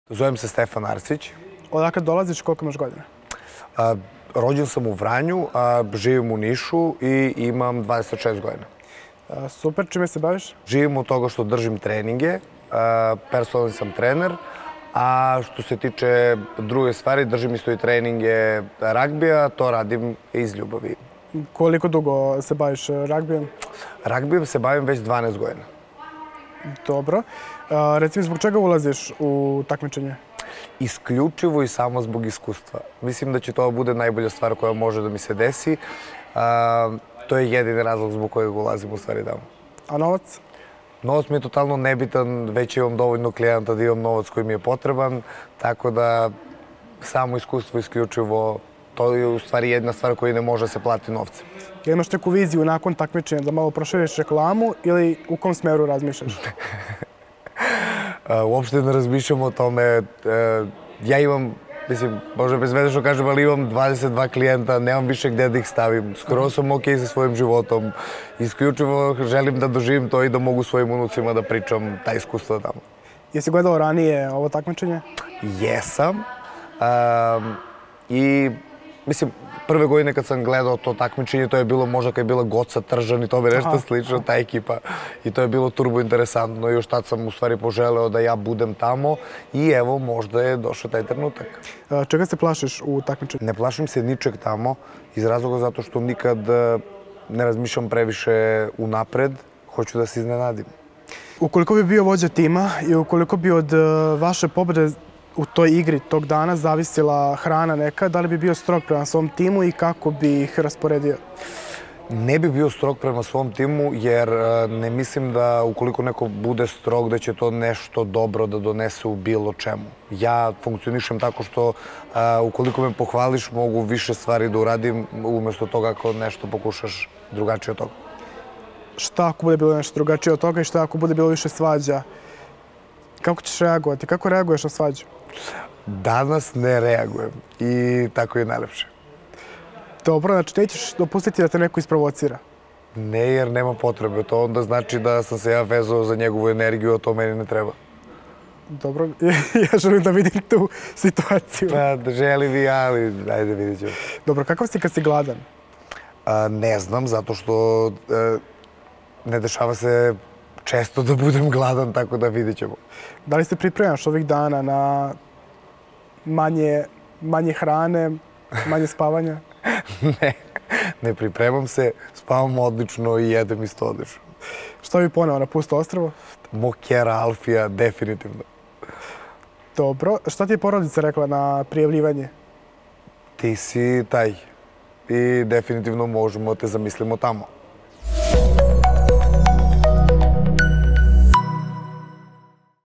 "To sam poželeo zbog Goce Tržan": Stefan Arsić večeras ulazi u Survivor
Stefan Arsić je poreklom iz Vranja, a već dugo godina živi u Nišu i ima 26 godina.

Stefan je personalni trener u teretani, a takođe drži treninge ragbija.

Kako je istakao u Survivor ulazi zbog iskustva koje novcem ne može da se plati.